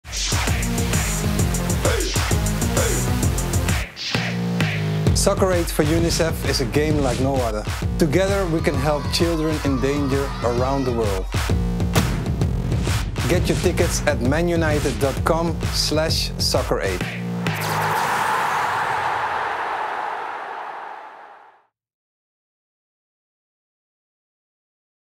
Barcelona legend Patrick Kluivert will make his Soccer Aid debut | Soccer Aid for Unicef
Welcome to team Usain's Patrick Kluivert.

Two teams. One goal. One unforgettable game of football.